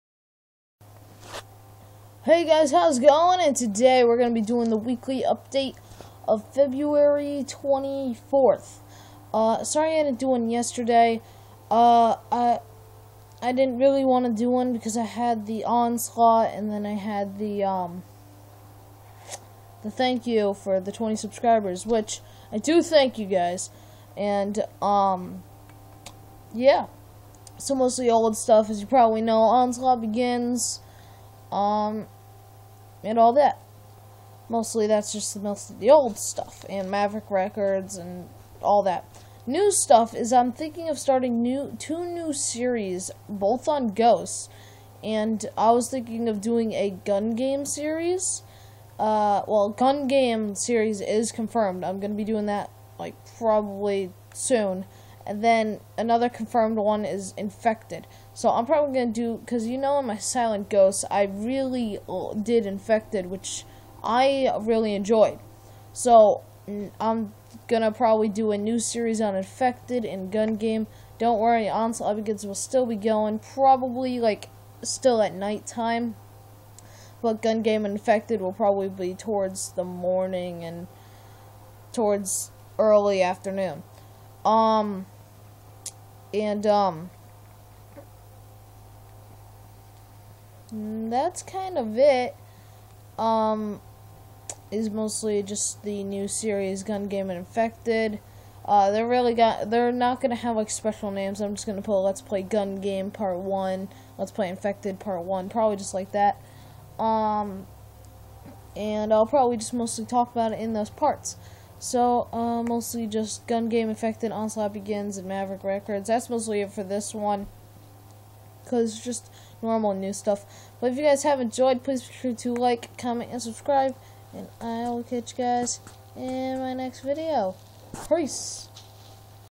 Weekly Update (February 24, 2014)
Onxlaught Begins, Maverick Records, 2 New Series